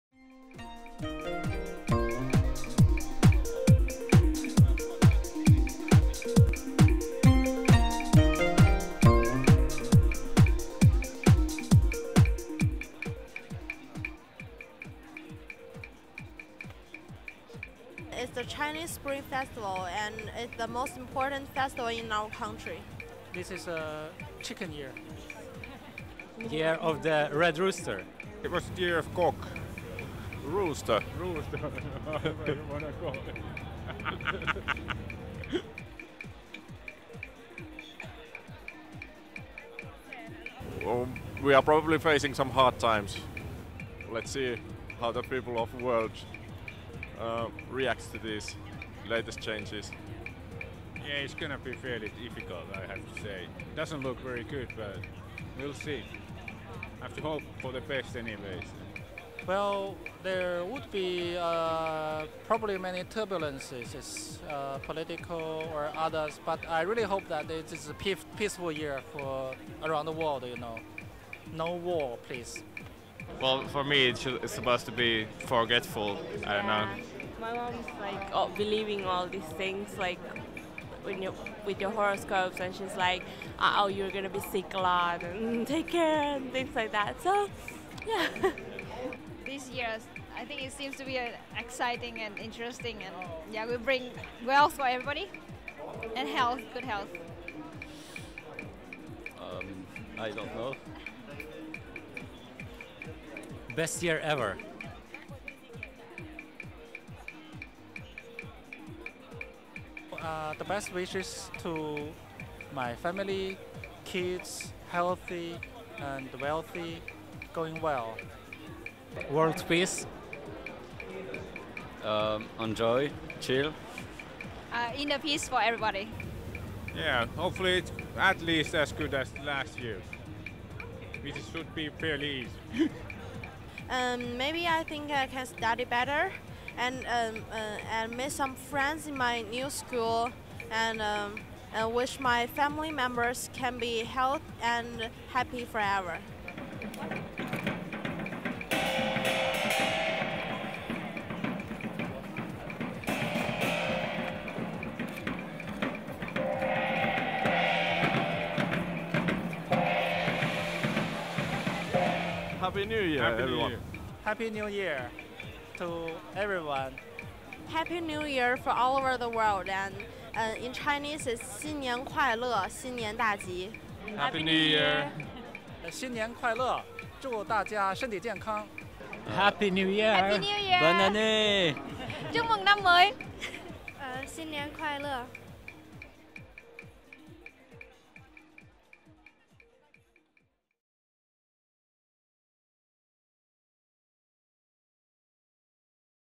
Chinese New Year 2017 - Helsinki, Finland (Year of the Rooster)
The beginning of a new lunar year – the Spring Festival – is the most important of Chinese festivals. All over the world, people gather to celebrate a fresh beginning with colourful decorations, tasty delicacies and time-honoured traditions.